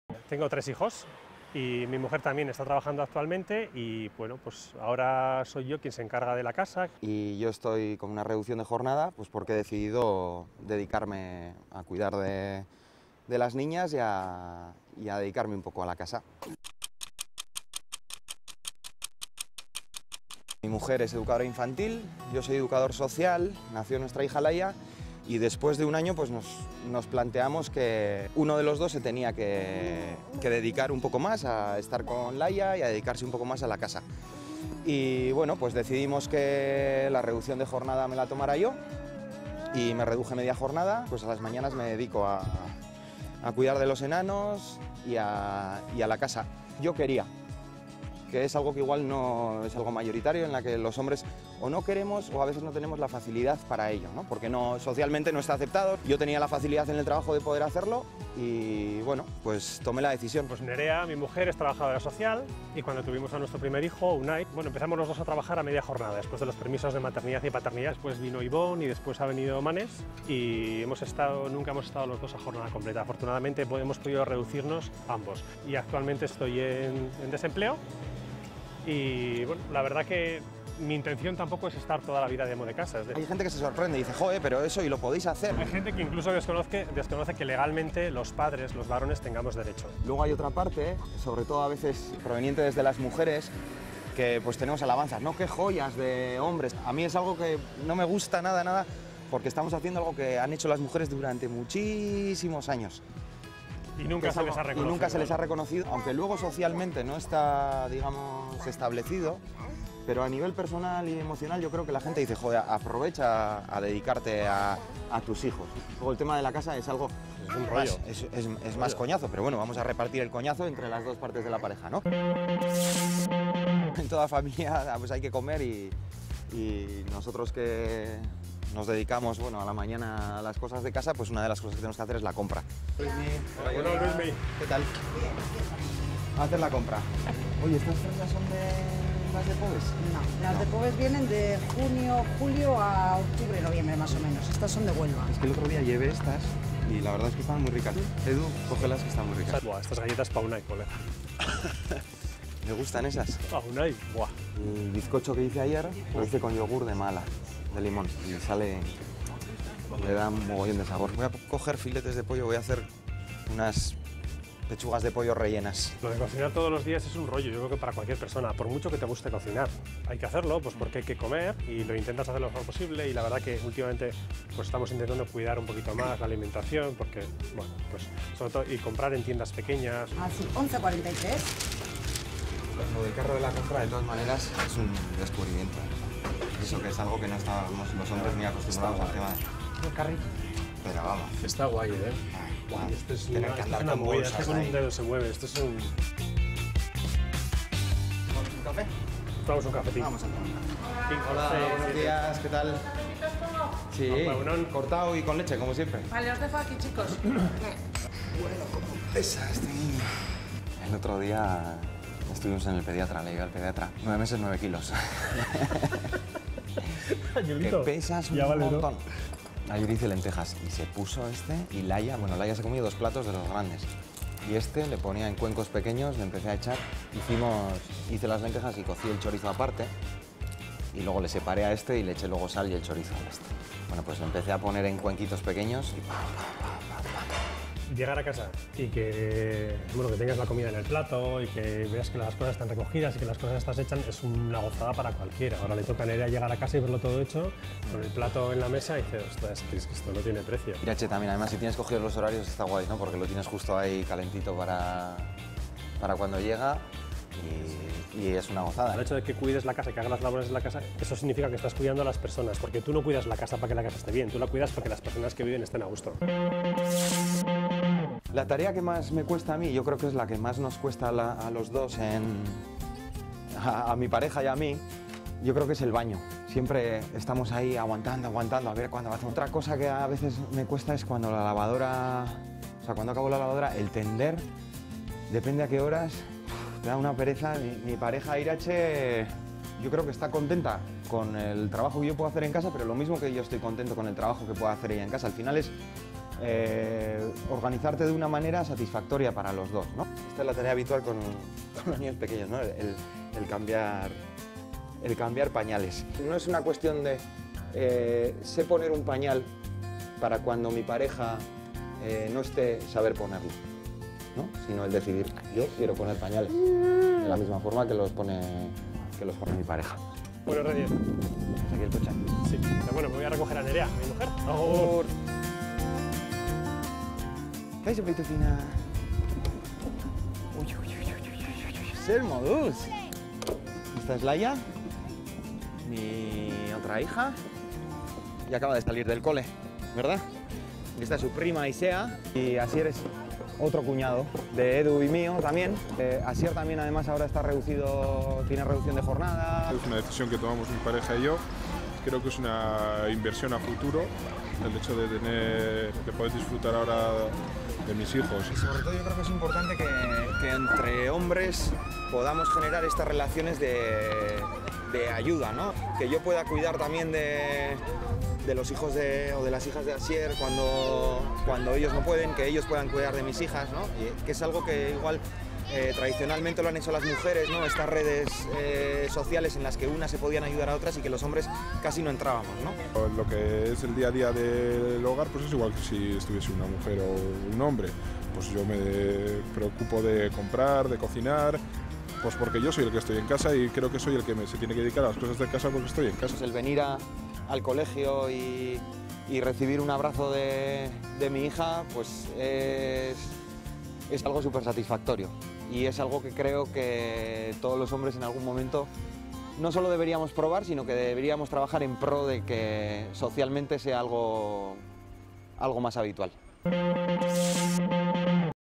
Cada vez hay más amos de casa
Diego optó por coger la jornada reducida para ocuparse de los niños. Eduardo, al quedarse en paro, decidió encargarse de la casa.